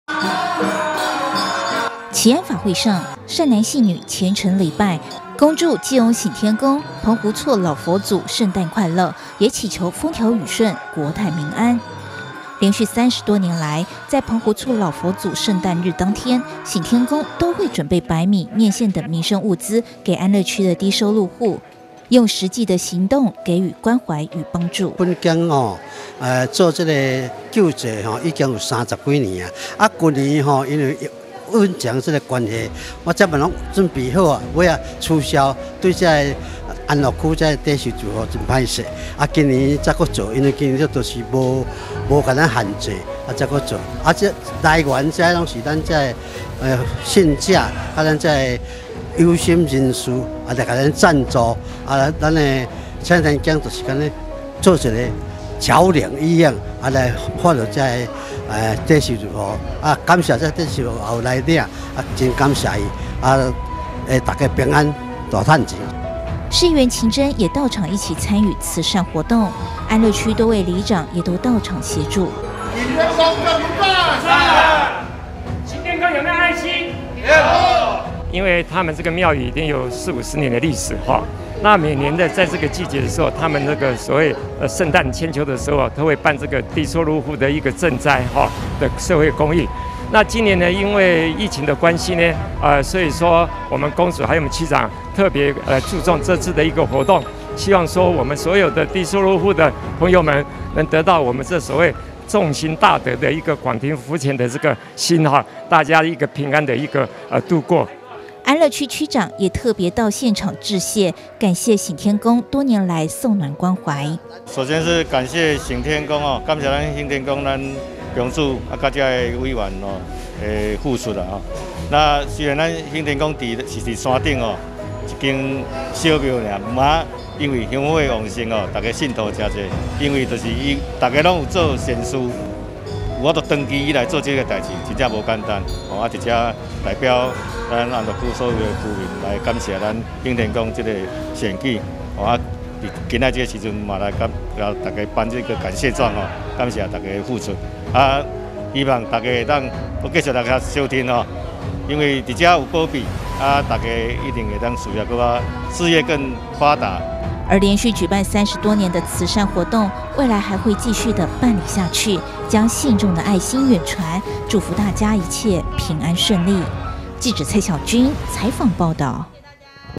慈善 送暖|物資 發送|醒天宮三十多年慈善送暖 贈低收入戶民生物資
(中嘉吉隆新聞 記者蔡曉君/基隆報導)基隆醒天宮澎湖厝老佛祖誕辰，辦理低收入戶發米濟助活動，三十多年來不間斷地送暖，安樂區長及多位地方民代也都到現場協助及感謝。

臉書追起來
Home 中嘉- 吉隆有線電視

趣聞不漏接
吉隆官網(中嘉新聞)

喜歡看電視?